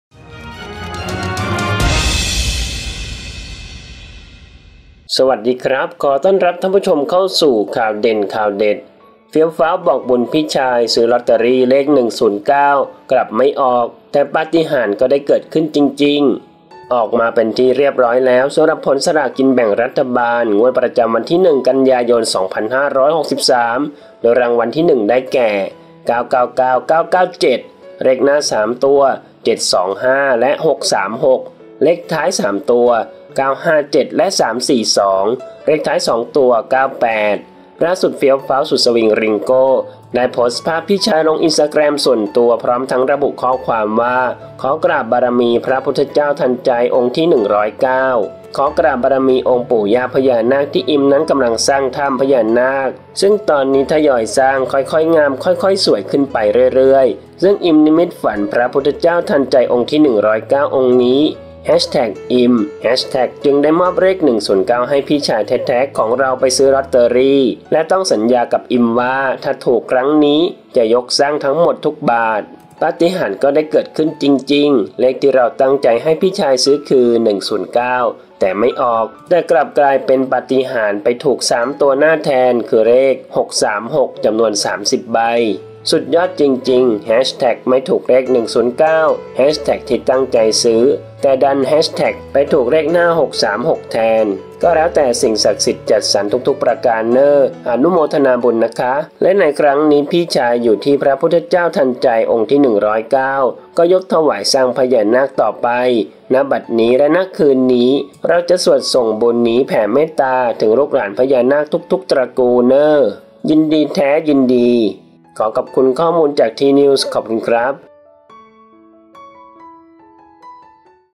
เฟี้ยวฟ้าว์ บอกบุญพี่ชาย ซื้อลอตเตอรี่เลข 109 กลับไม่ออก แต่ปาฏิหาริย์ก็ได้เกิดขึ้นจริงๆ
ออกมาเป็นที่เรียบร้อยแล้ว สำหรับผลสลากกินแบ่งรัฐบาลงวดประจำวันที่ 1 กันยายน 2563

โดยรางวัลที่ 1 ได้แก่ 999997
เลขหน้า 3 ตัว  725 ,636
เลขท้าย 3 ตัว 957, 342
เลขท้าย 2 ตัว 98

ล่าสุด เฟี้ยวฟ้าว์ สุดสวิงริงโก้ ได้โพสต์ภาพ พี่ชายลงอินสตาแกรมส่วนตัวพร้อมทั้งระบุข้อความว่า "ขอกราบบารมี พระพุทธเจ้าทันใจองค์ที่109
ขอกราบบารมีองค์ ปู่ย่าพญานาค ที่ อิม นั้นกำลังสร้างถ้ำ..พญานาค ซึ่งตอนนี้ ทะยอยสร้าง ค่อยๆ งามค่อยๆ สวยขึ้นไปเรื่อยๆ ซึ่ง อิม นิมิตฝัน
พระพุทธเจ้าทันใจองค์ที่109 องค์นี้ อิม.. จึงมอบเลขนี้ 109 ให้พี่ชายแท้ๆ ของเราไปซื้อลอตเตอรี่ และ ต้องสัญญากับ อิม ว่าถ้าถูก ครั้งนี้ จะยกสร้างทั้งหมดทุกบาท

ปาฏิหาริย์ก็ได้เกิดขึ้นจริงๆ เลขที่เราตั้งใจให้พี่ชายซื้อ คือ 109 แต่ไม่ออก 109 ไม่ออก แต่กลับกลายเป็นปาฏิหาริย์ ไปถูก 3 ตัวหน้าแทน คือเลข 636
จำนวน 30 ใบ

สุดยอดจริงๆ ไม่ถูกเลข 109 ที่ตั้งใจซื้อ แต่ดัน ไปถูกเลขหน้า 636 แทน ก็แล้วแต่ตามสิ่งศักดิ์สิทธิ์จัดสรรทุกๆประการเน้อ อนุโมทนาบุญนะคะ
และในครั้งนี้ พี่ชาย อยู่ที่ พระพุทธเจ้าทันใจองค์ที่109 ก็ยกถวาย สร้างพญานาค ต่อไป ณ บัดนี้ และ คืนนี้เราจะสวดส่งบุญนี้แผ่เมตตา ถึงลูกหลานพญานาคทุกๆ ตระกูล..เน้อ ยินดีแท้ ยินดี"